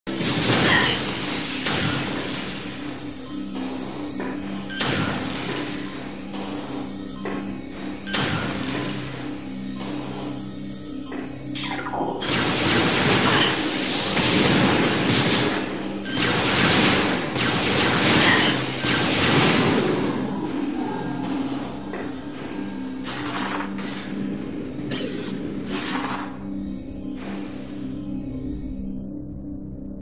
Quarantine Access A SW (Remastered)
more precise setup with the turret. you want to stand where i am in video and shoot and walk backward (not sure if walking back needed but i got most good results with it). this setup seemed to let me get stuck to perform the repo. everything else is the same.